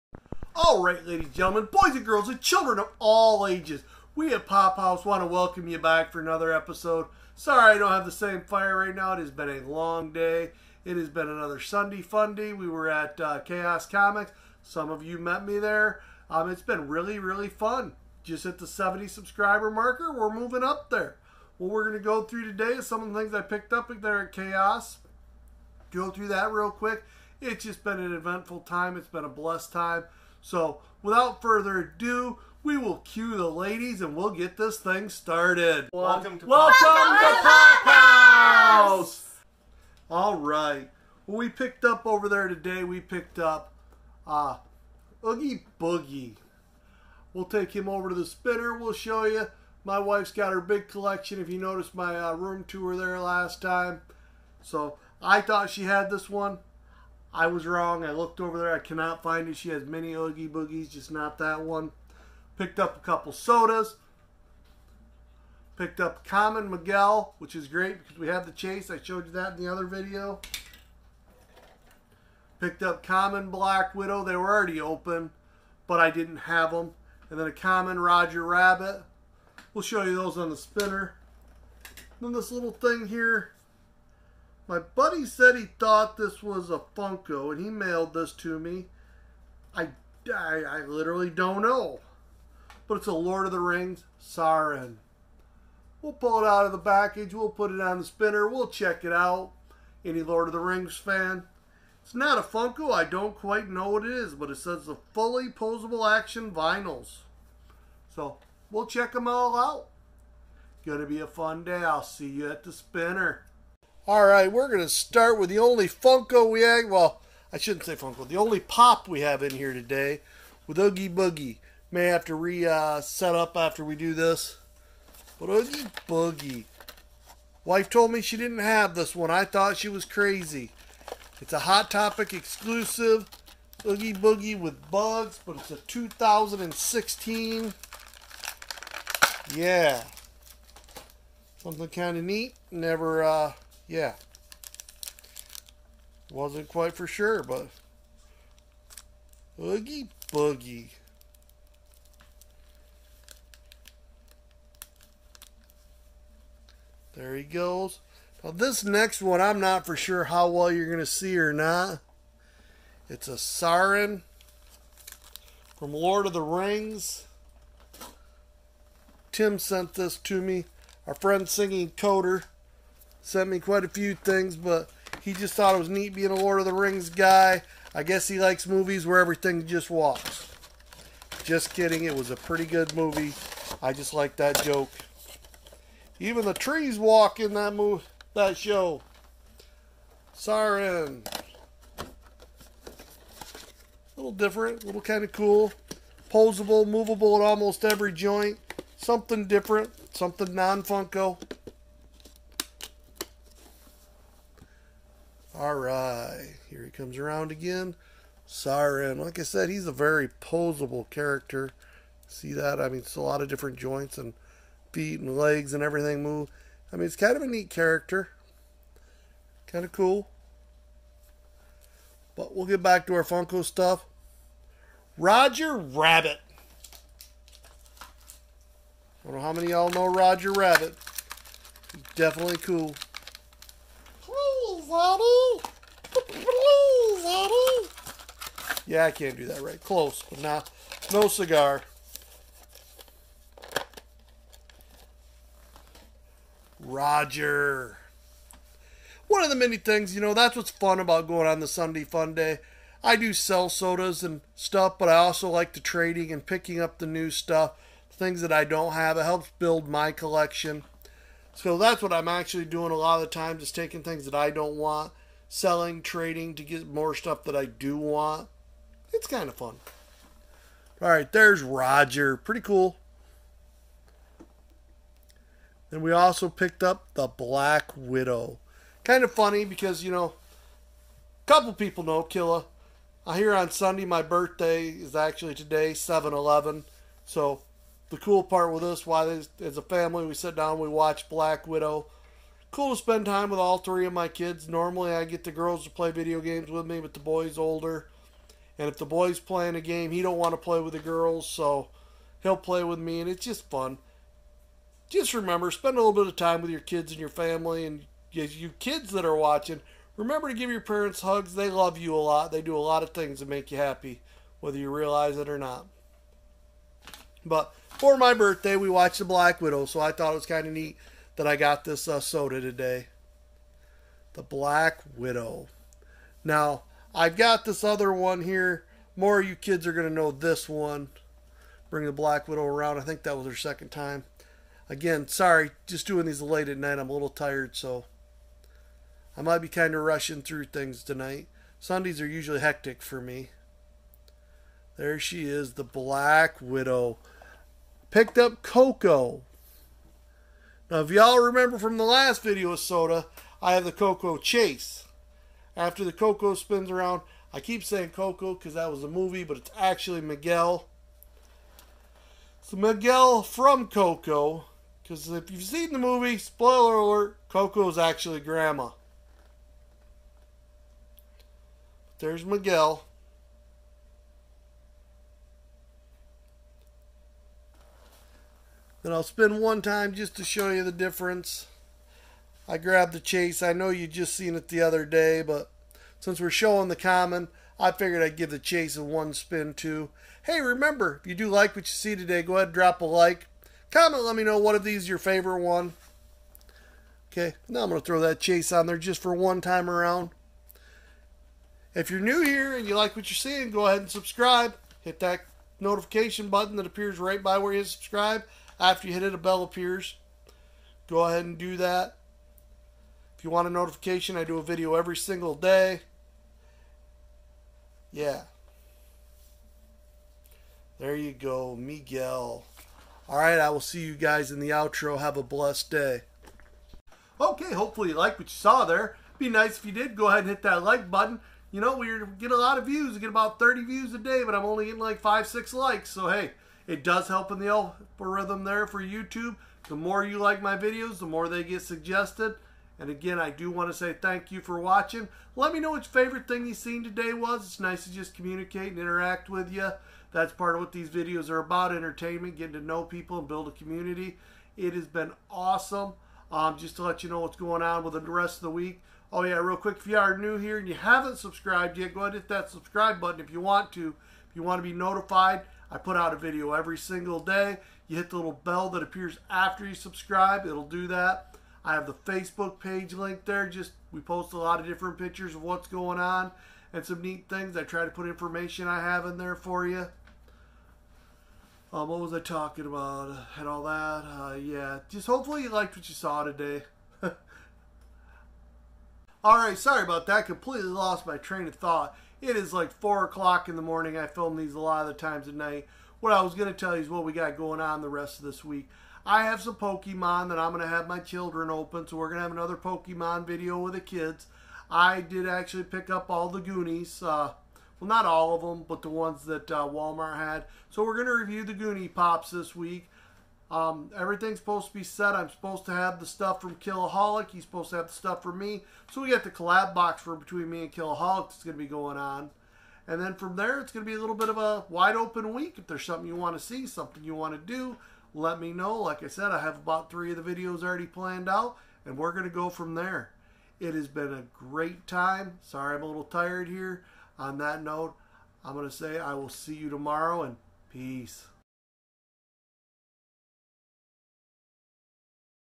Sunday Funday Pick ups
Pophouse Facebook

Nightmare before Christmas Funko pop

Disney Funko pop

Roger Rabbit Funko Soda

Black widow Soda

Coco Soda